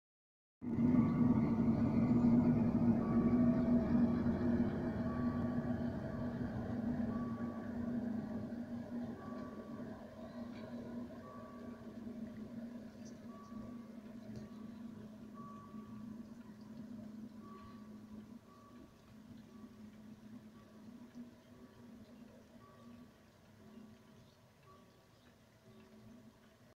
2016-12-14 OHE DL 1500 04 passiert Neetze
Tags zuvor zwar noch darüber gelesen, aber doch erfolgreich verdrängt überraschte mich DL 1500 04 dann eiskalt, so dass nur noch der Schnellschuss aus dem Fenster möglich war.
Die OHE Lok war zur Instandsetzung im Bleckeder Bleckwerk gewesen und wurde nun an den neuen Eigentümer überführt.